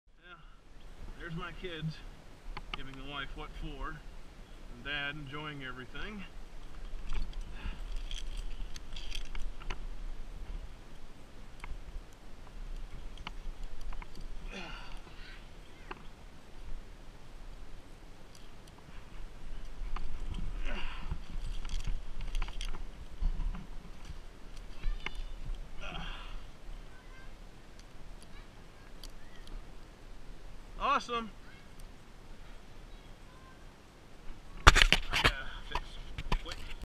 cleaning out the tree
had a huge snag get blown into it the other day. about a 105 hickory. I went up after we got in pulled out and cleaned out the branches it broke